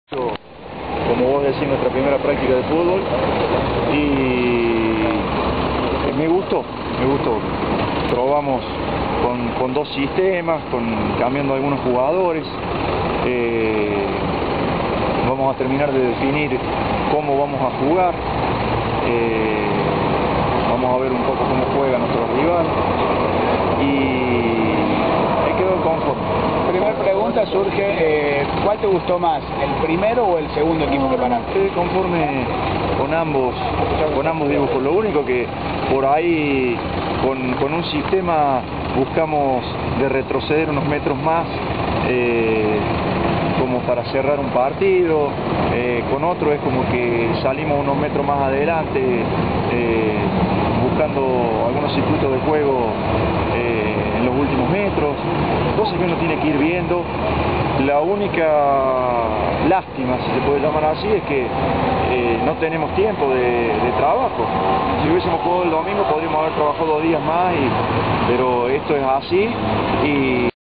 NOTA CON ARZUBIALDE
KUEGO DE LA PRACTICA DE FUTBOL , HECTOR ARZUBIALDE EXPLICA LOS DOS SISTEMAS DE JUEGO ESBOZADOS